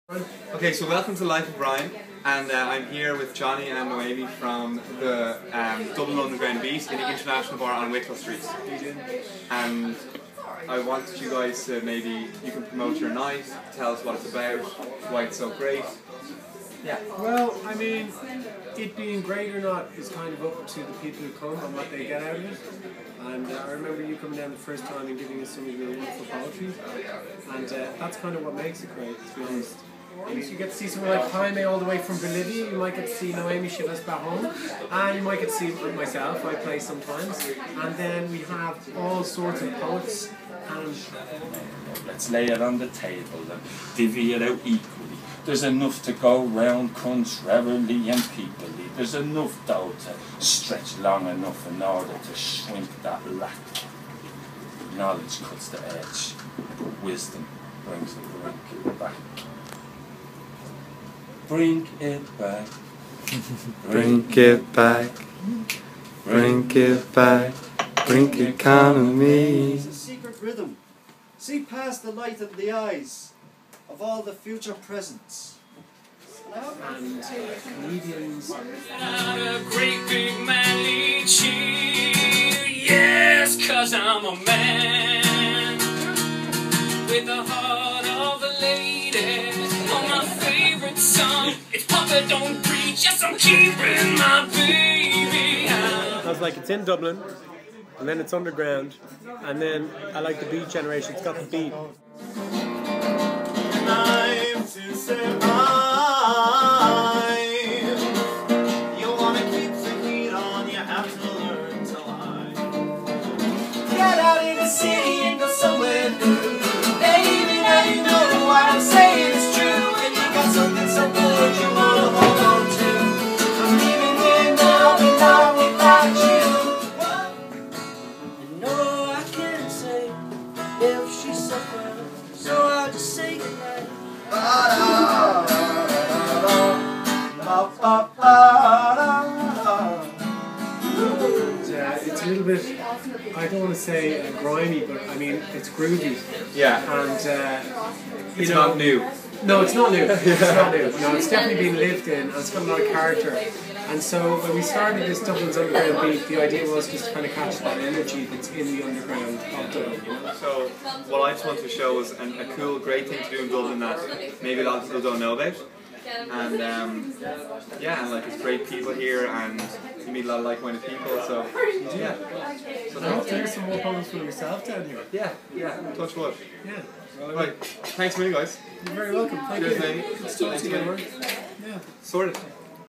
Ireland is Great - Dublin's Underground Beat (D.U.B) At the International Bar
D.U.B is an open-mike night in the basement of the International Bar on Wicklow street. There is a great, cosy atmosphere where the crowd always respects each performer and enjoys original music and poetry from Dublin's diverse range of talent. Between 30-50 people usually gather over a bowl of Irish stew, a pint of Guinnes or all (or neither) of the above and experience something different in the heart of Dublin.